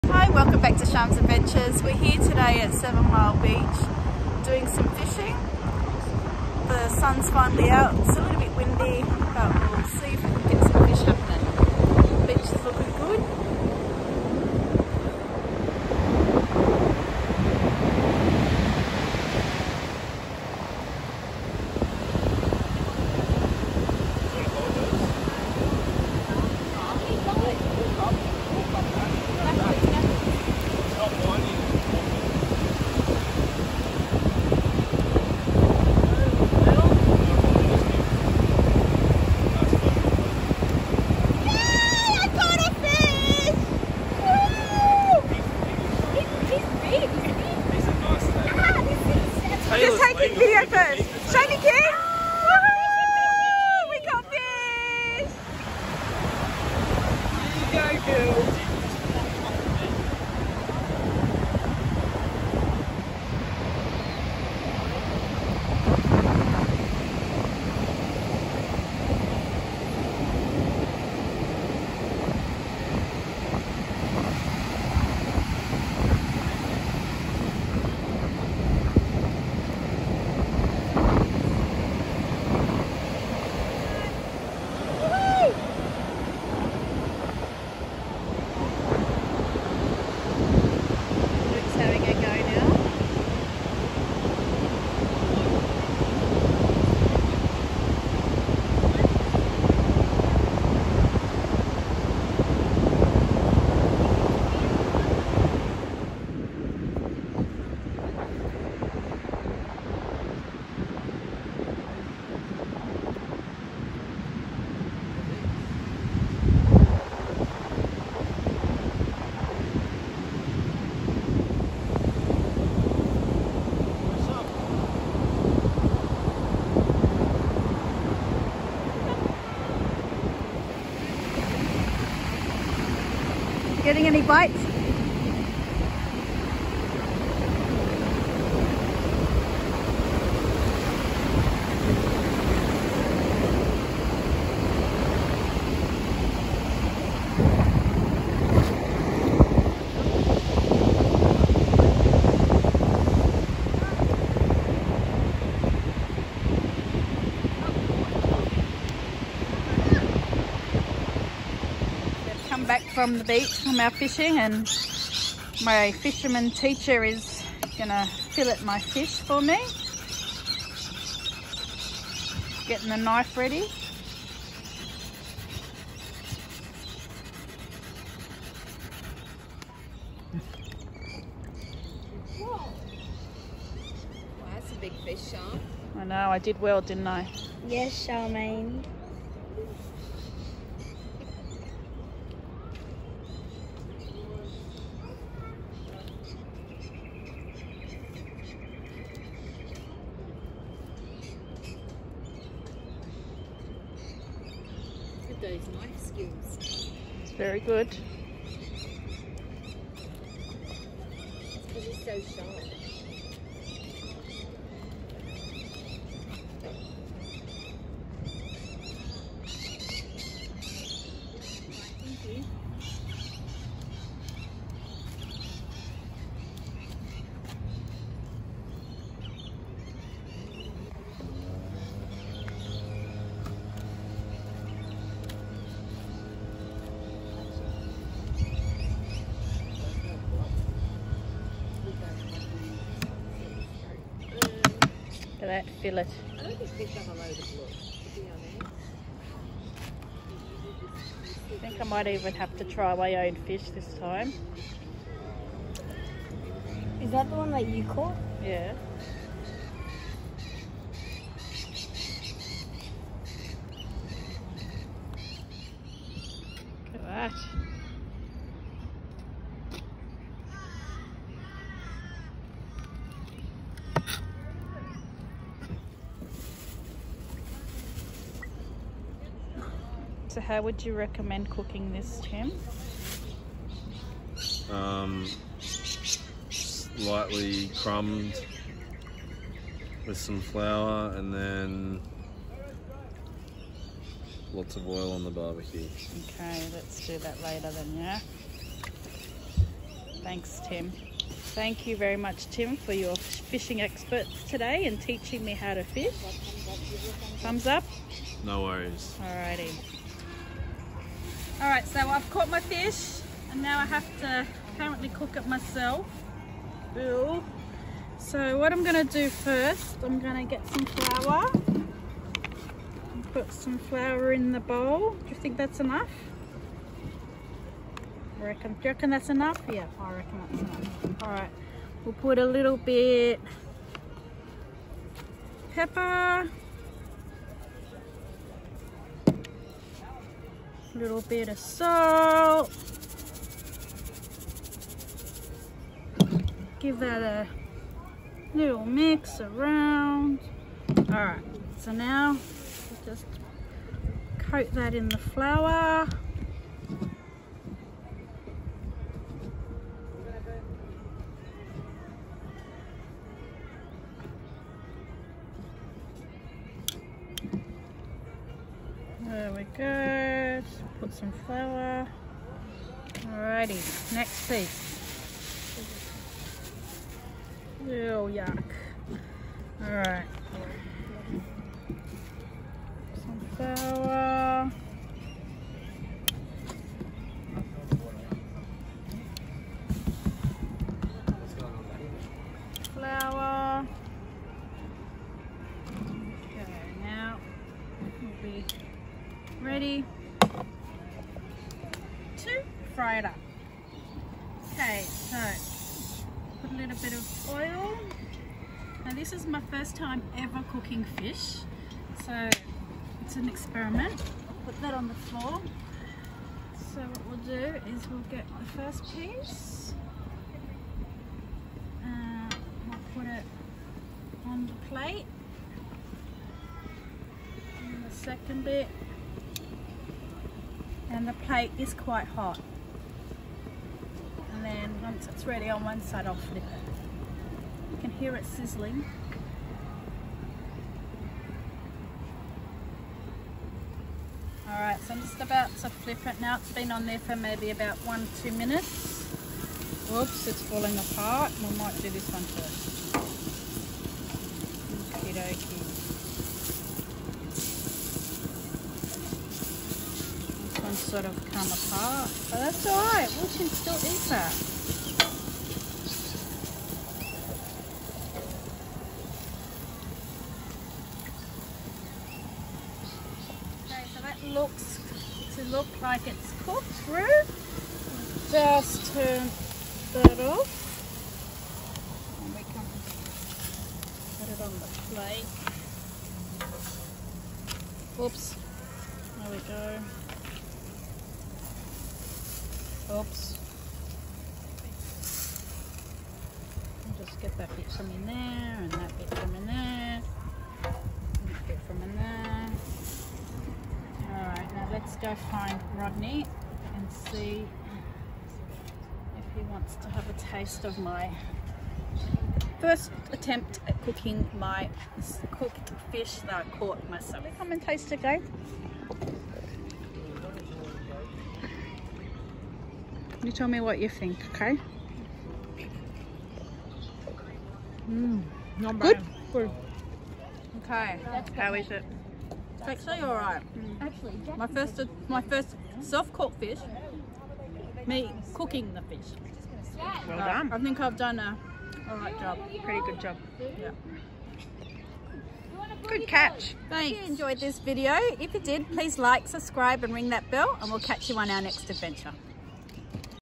Fishing At Seven Mile Beach Gerroa NSW
We spent the Easter week at Seven Mile Beach in Gerroa NSW
Camping and fishing with family and friends. I caught a fish and cooked it too.
Those who know me know that I don’t eat fish or cook fish but I had to try it.